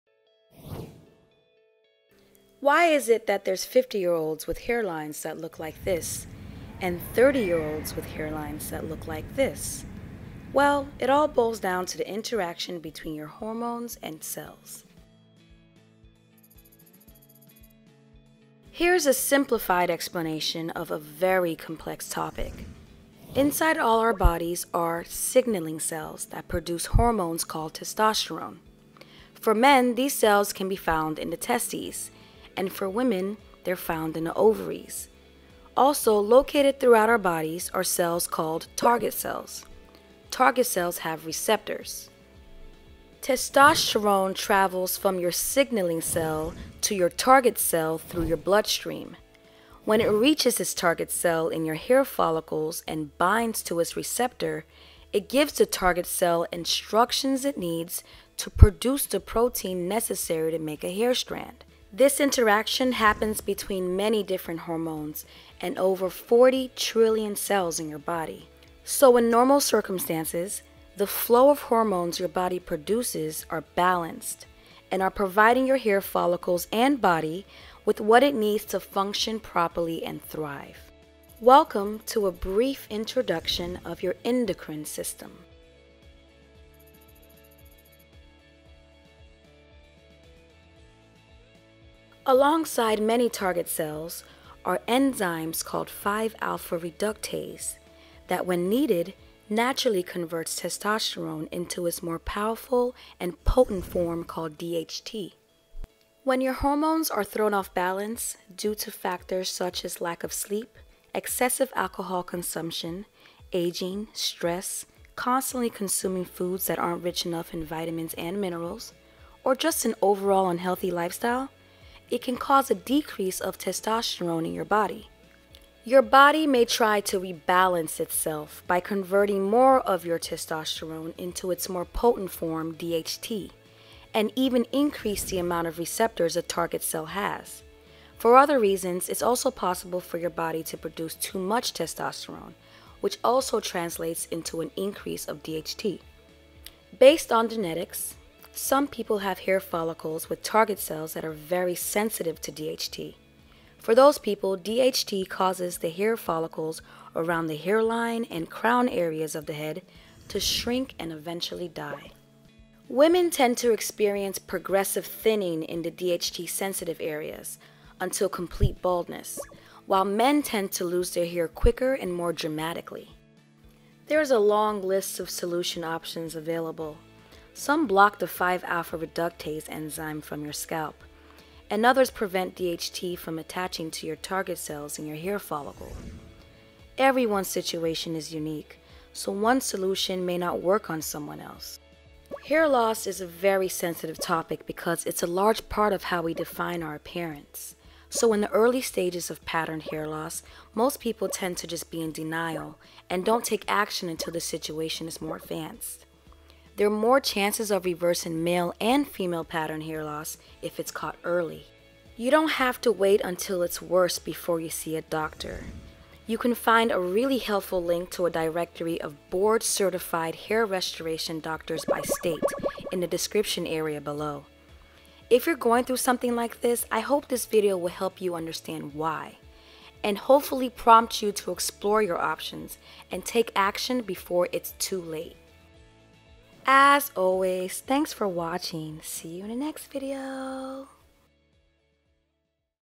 Causes of Male & Female Pattern Baldness
Hormonal imbalance is the main culprit of pattern hair loss, and it’s also related to your genes. Still, there’s higher probability of reversing it if it’s caught early.

📗 REFERENCE 📗